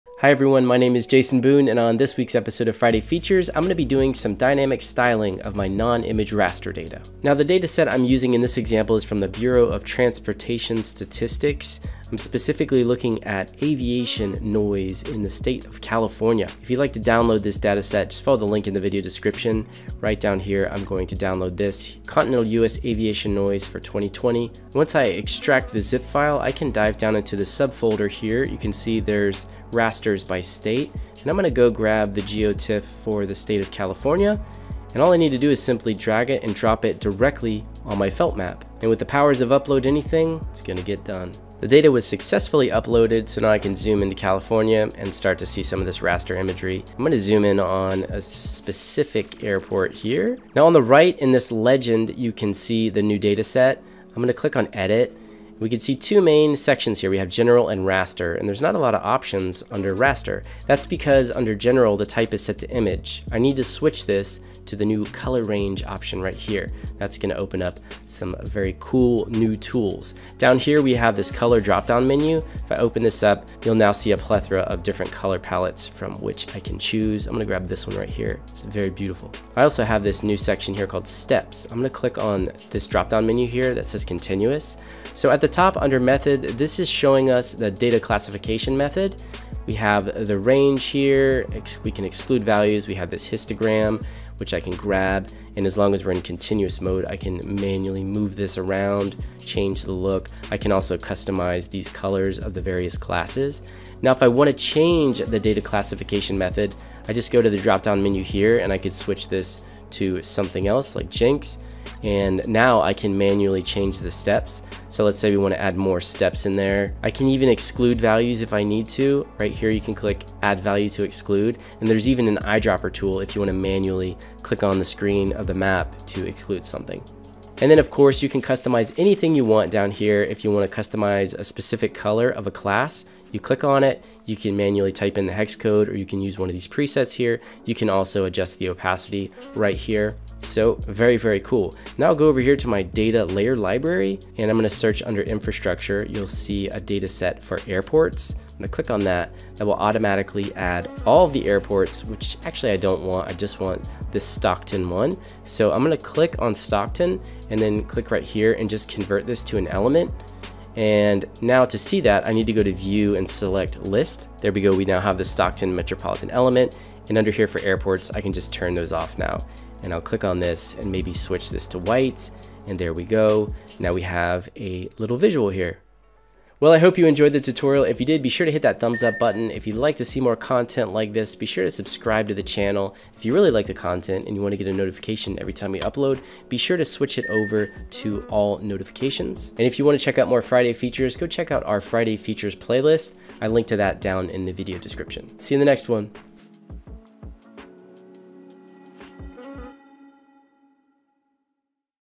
Stylize Your Rasters with NEW Advanced Color Controls🎨
Styling your data with Felt's new raster styling tools is incredibly powerful and easy. 💪Check it out!

0:00 Intro
0:07 Get the Data
0:28 Upload the Data
0:49 Switch to Color Range
1:19 Style!
2:21 Add Additional Data Layers
2:53 The Final Visual